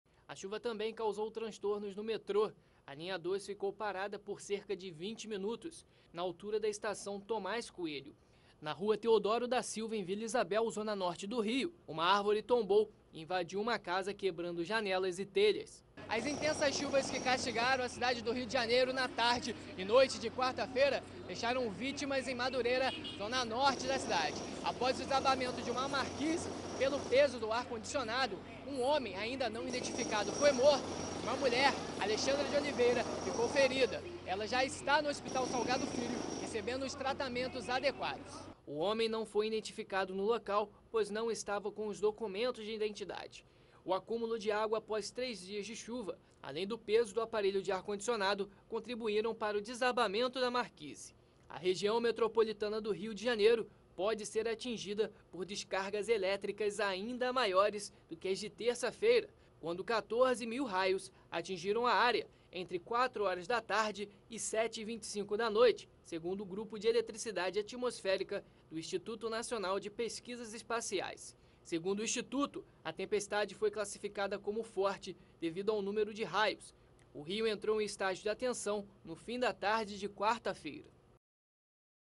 Fortes chuvas atingem o Rio de Janeiro e deixa a cidade em estado de atenção
A forte chuva que caiu sobre o Rio de janeiro, no início da noite de quinta-feira, causou transtornos em vários bairros. Árvores caíram em pelo menos 13 pontos, principalmente nas zonas norte e oeste. Na região, moradores também reclamam de falta de energia elétrica. Uma pessoa morreu em um desabamento.